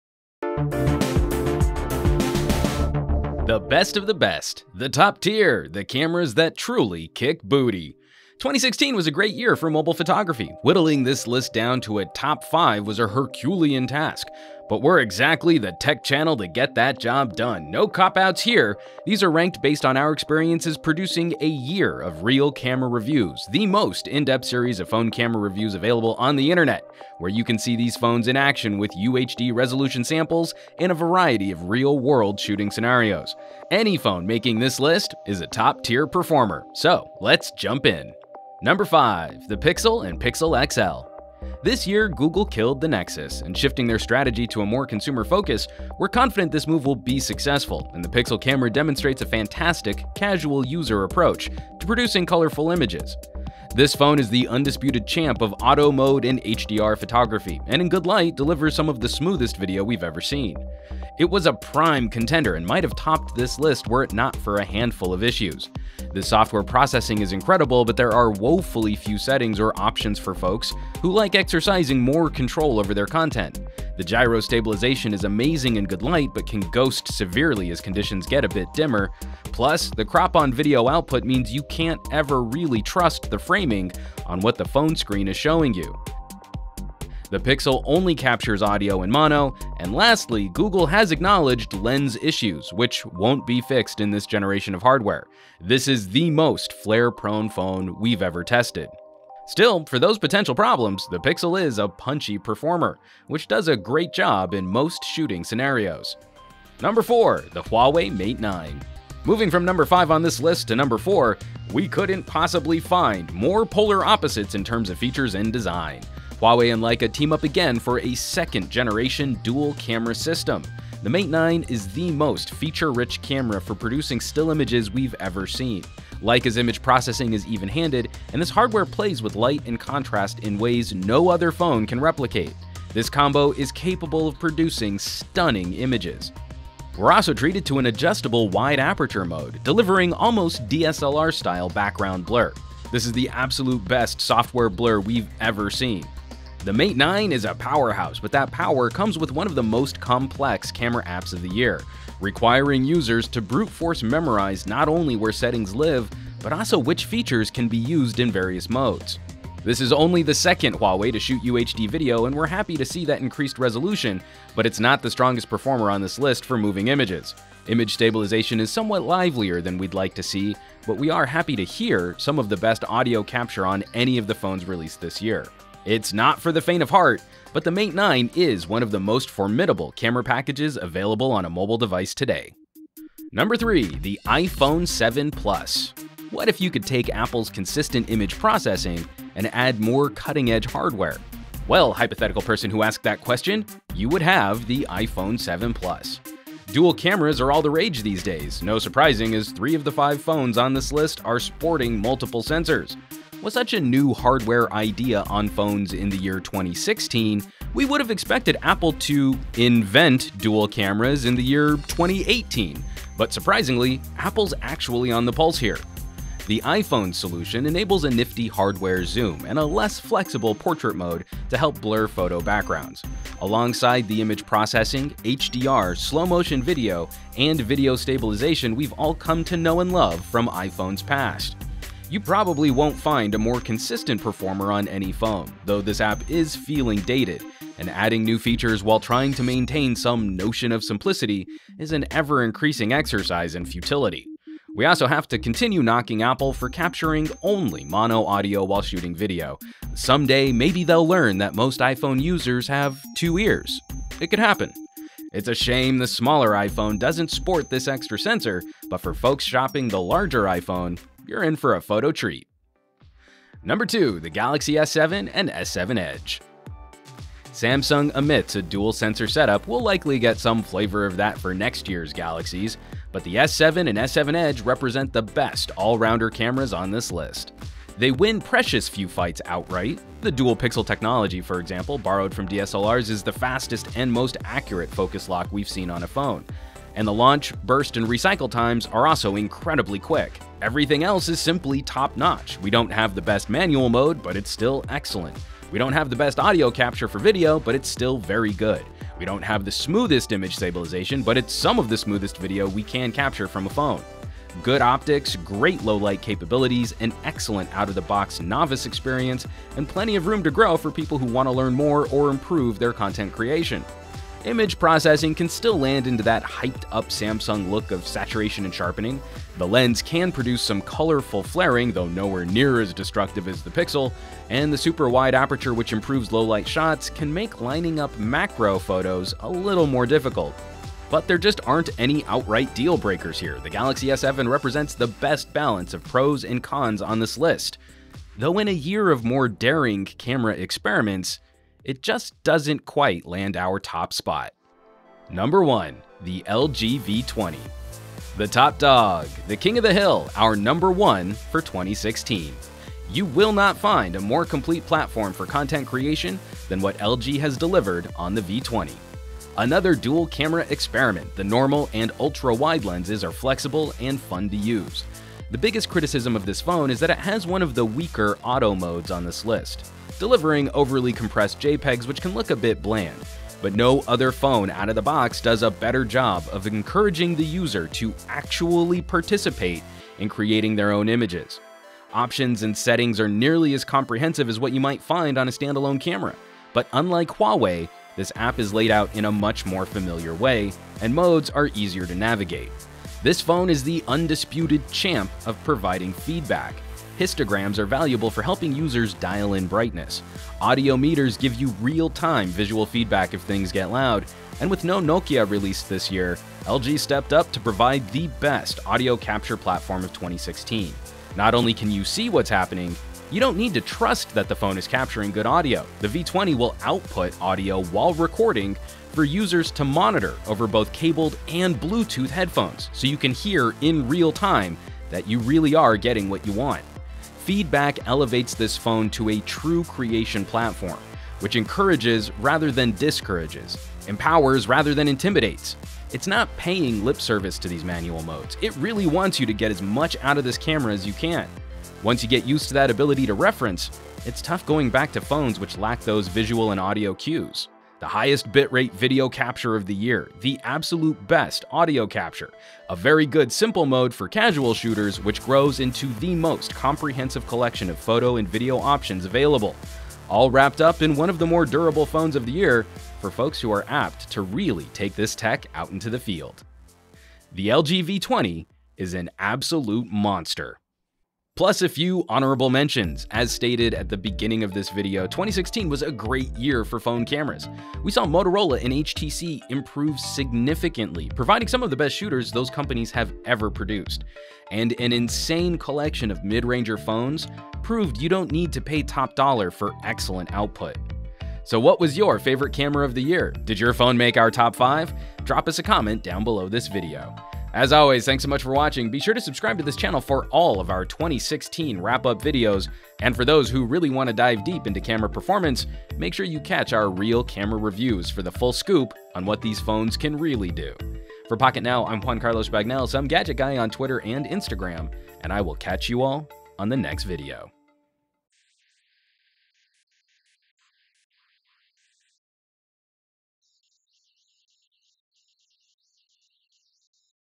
Top 5 Smartphone Cameras of 2016: Best mobile photos and video! | Pocketnow
Next for our 2016 wrap up videos, we're judging the best phone cameras of the year! After producing a year of Real Camera Reviews, here are the best of the best. The best photos. The best video. The best camera apps. If cameras are a killer feature, then these phones are the absolute content creation devices of 2016!